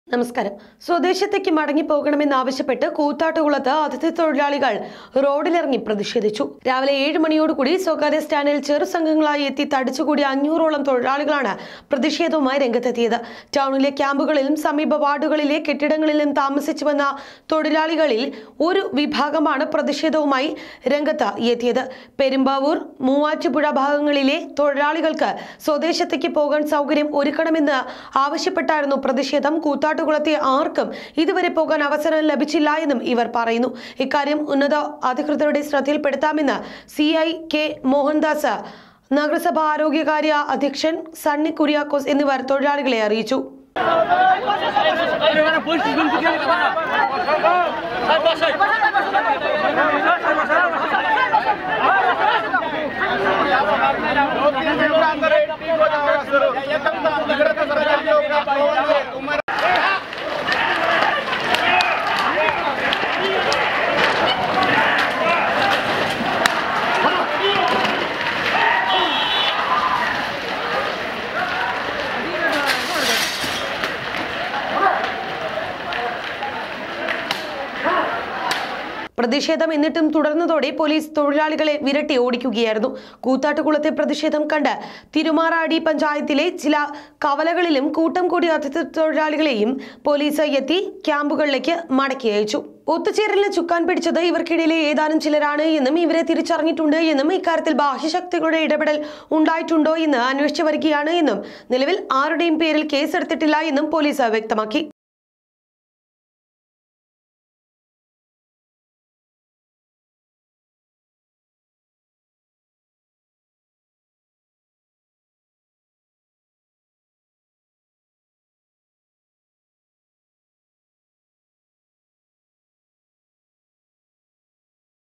അതിഥി തൊഴിലാളികളുടെ പ്രതിഷേധം.. വിരട്ടിയോടിച്ച് പൊലീസ്l migrant labours stage protest
സ്വദേശത്തേക്ക് മടങ്ങിപ്പോകണം എന്നാവശ്യപ്പെട്ട്കൂത്താട്ടുകുളത്ത് അതിഥി തൊഴിലാളികളൾ റോഡിലിറങ്ങി.രാവിലെ 7 മണിയോടെ സ്വകാര്യ സ്റ്റാന്റിൽ ചെറുസംഘങ്ങളായി എത്തി തടിച്ചുകൂടിയ അഞ്ഞൂറോളം തൊഴിലാളികളാണ് പ്രതിഷേധം നടത്തിയത്